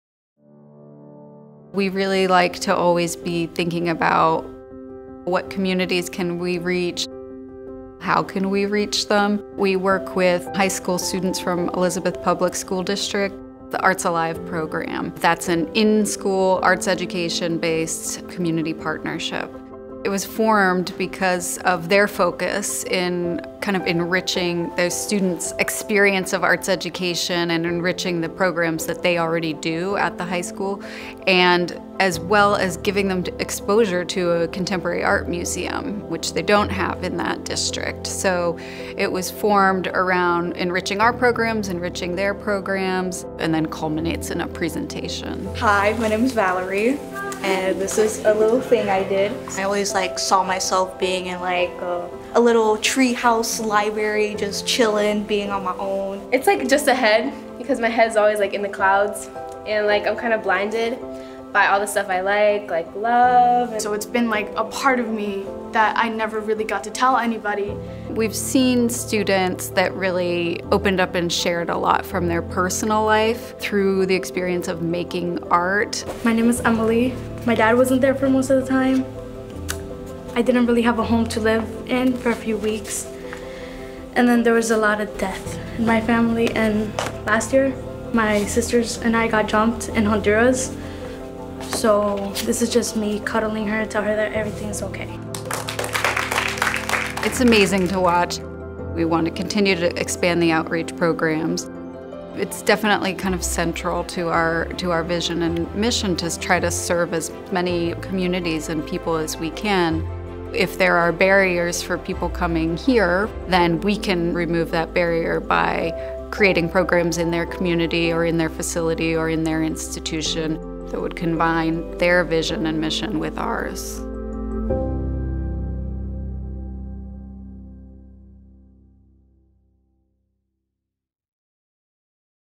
Visual Arts Center of New Jersey - Arts Alive Initiative
Since 2009, the Arts Alive Initiative has brought 300 students per year from Jefferson Arts Academy to the Art Center for an in-depth sequential learning program that expands their knowledge and understanding of contemporary art. The students visit for three consecutive weeks during each exhibition season and create art projects that apply ideas and themes from the Art Center’s exhibitions.